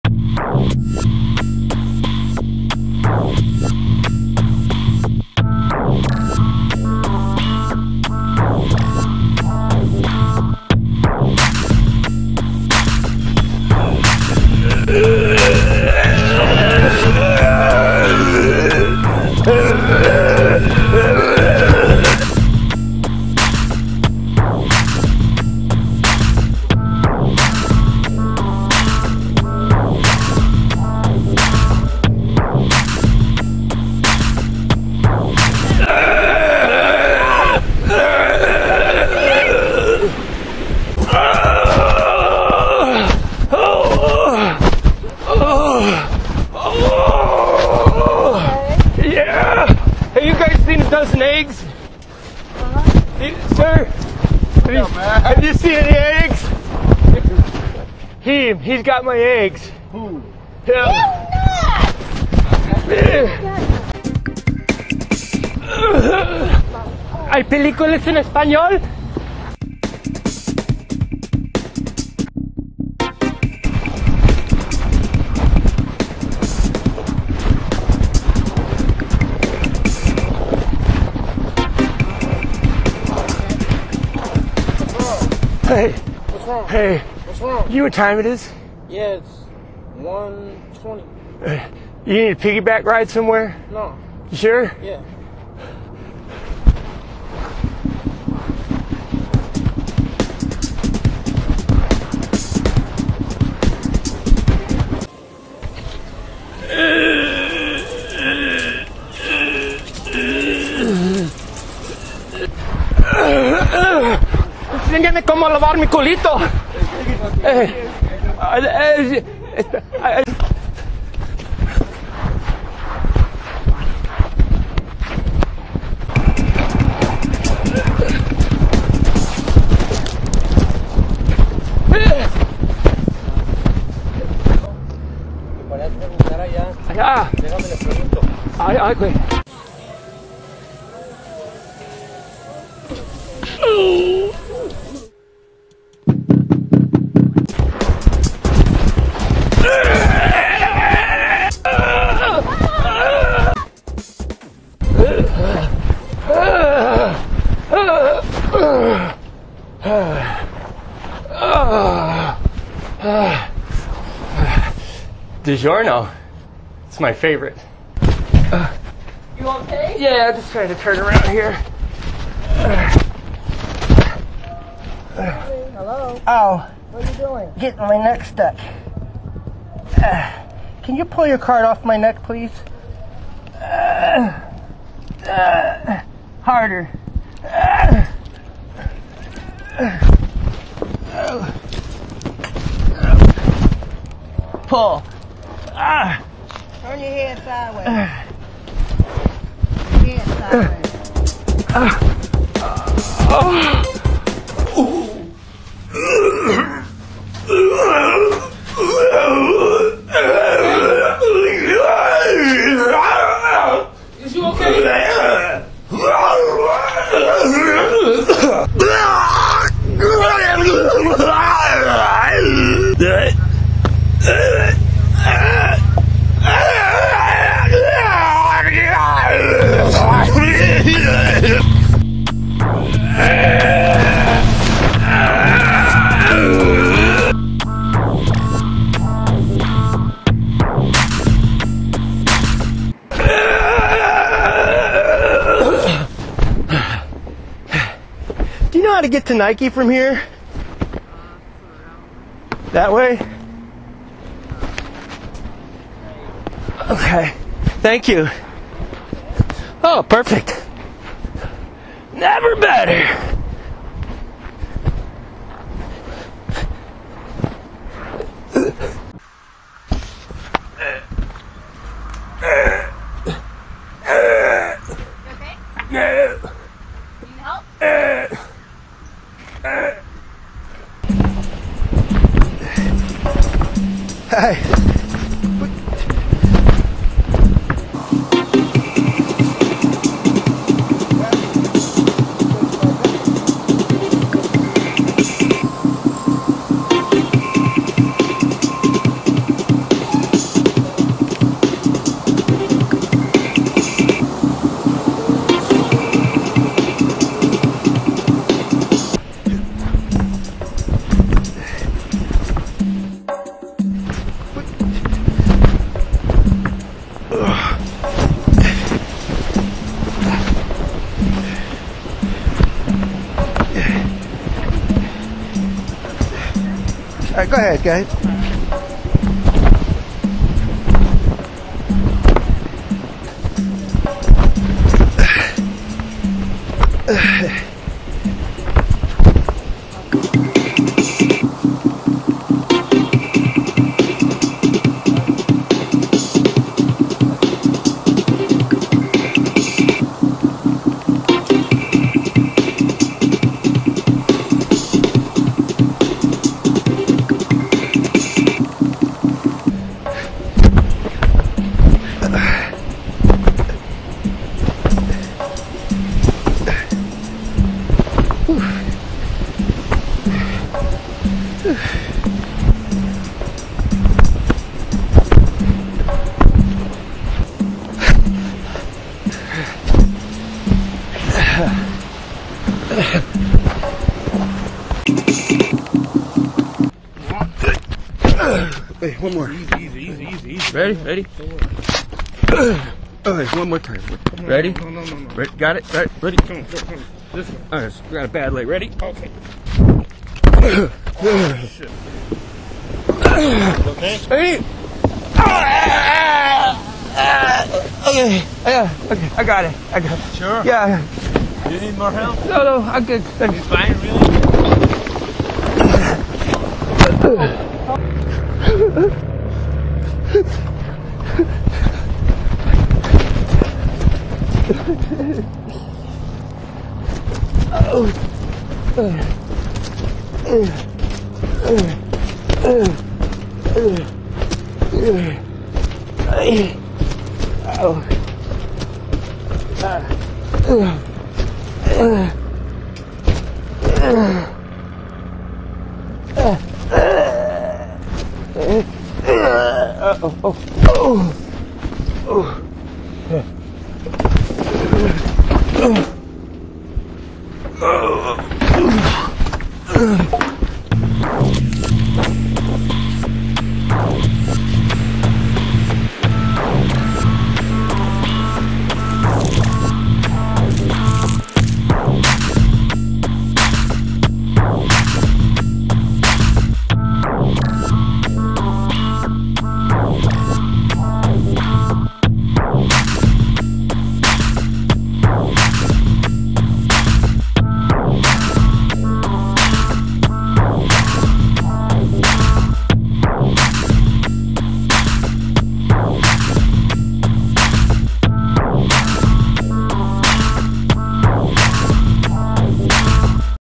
MENTALLY CHALLENGED ZOMBIE - Crawling Zombie Prank!!!
For my Halloween video this year I decided to do another Zombie Crawling prank video.  I scare most of my prank victims with this prank. I just filmed some brand new scenes but it wasn't long enough to make a full video, so I decided to mix it with some older video clips to make a nice full video.
  My "Zombie Crawl" video series was originally called "Sliding"...I was never trying to imitate a zombie, but many of my subscribers said I looked like a mentally challenged zombie crawling and flopping around on the ground, so I decided to change the name of the series and the theme.